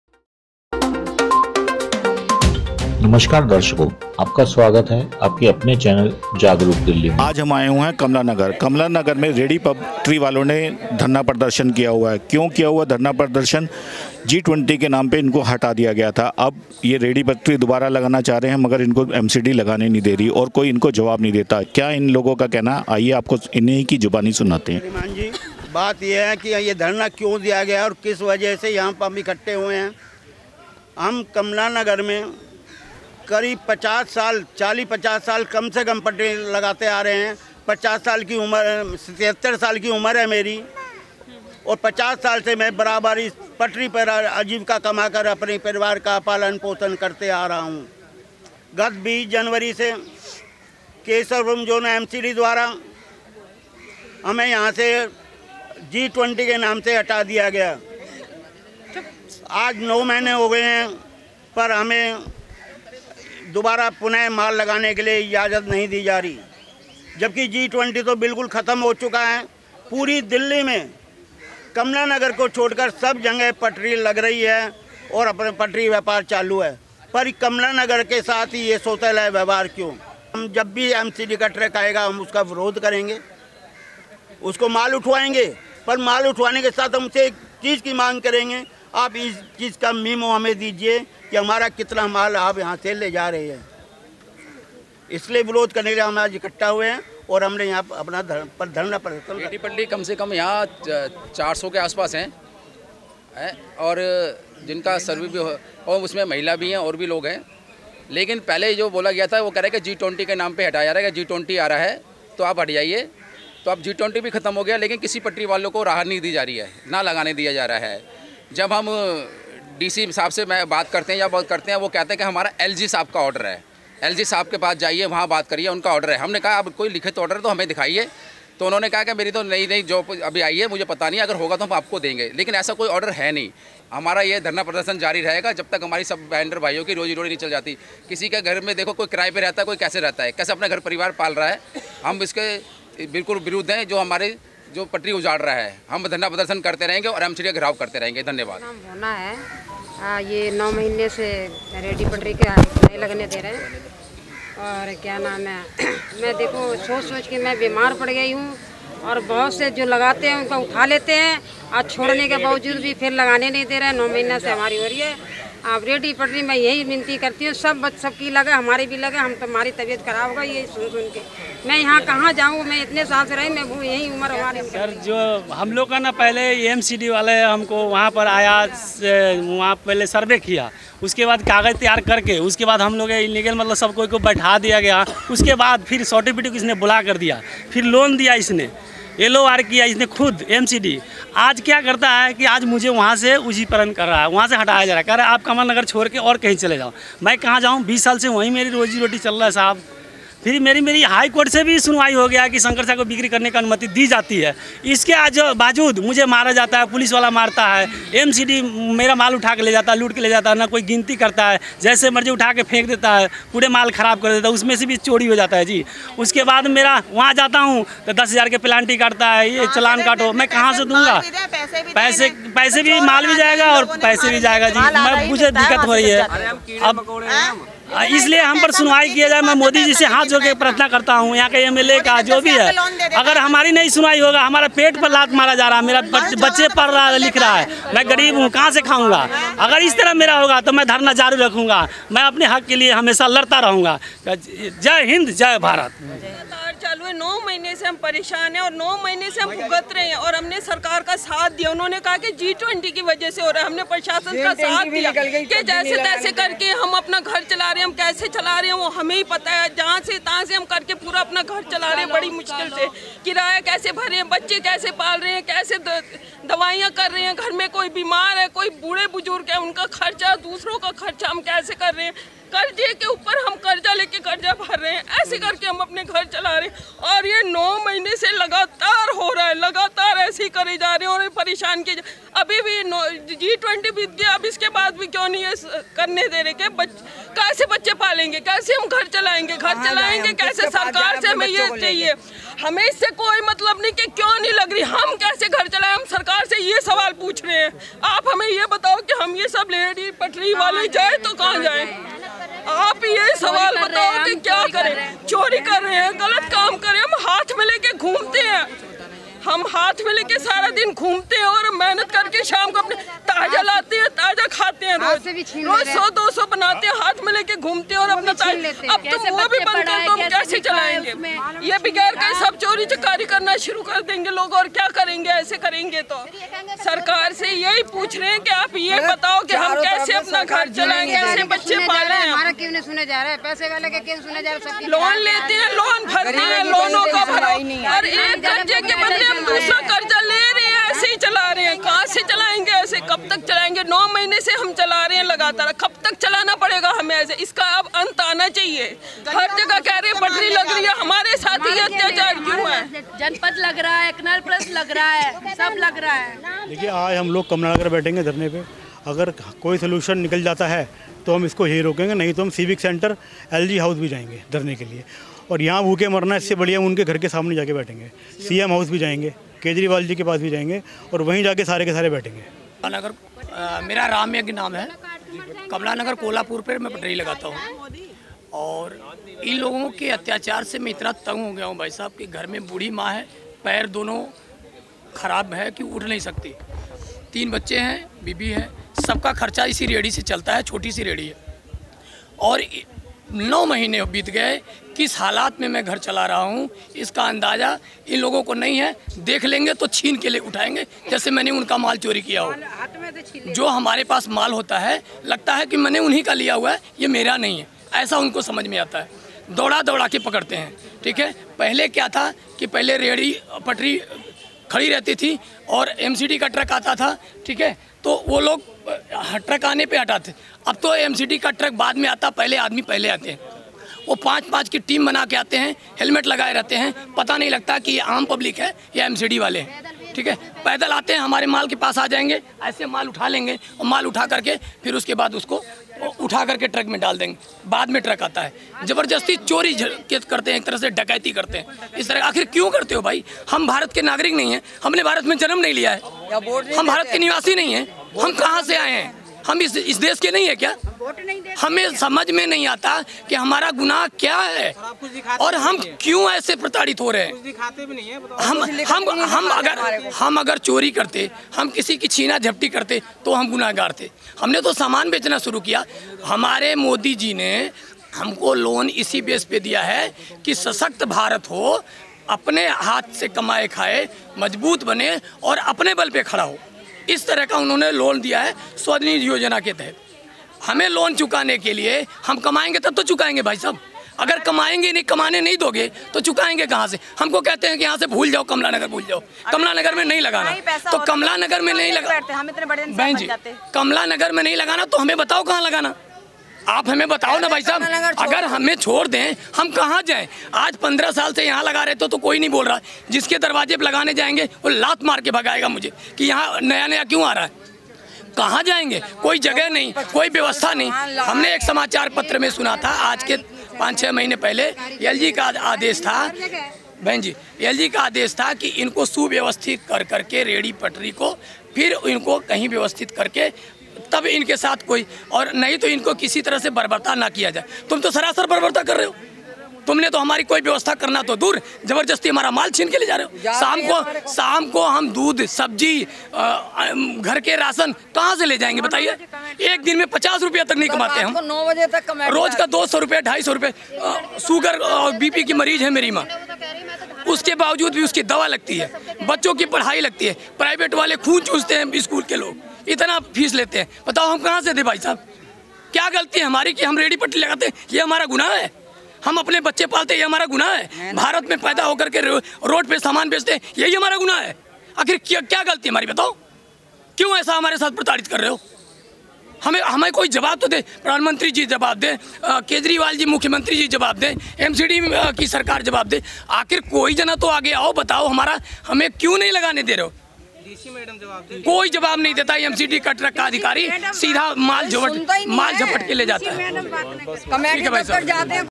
सरकार के खिलाफ 400 रेहड़ी पटरी वाले बैठे धरने पर
कमला नगर के रेहड़ी पटरी वालों का कहना है सरकार ने पहले हमें लोन दिया उसके बाद अब रोजगार छीन लिया । कैसे करेंगे लोन की भरपाई। MCD रोज आती है और समान लेकर चली जाती है।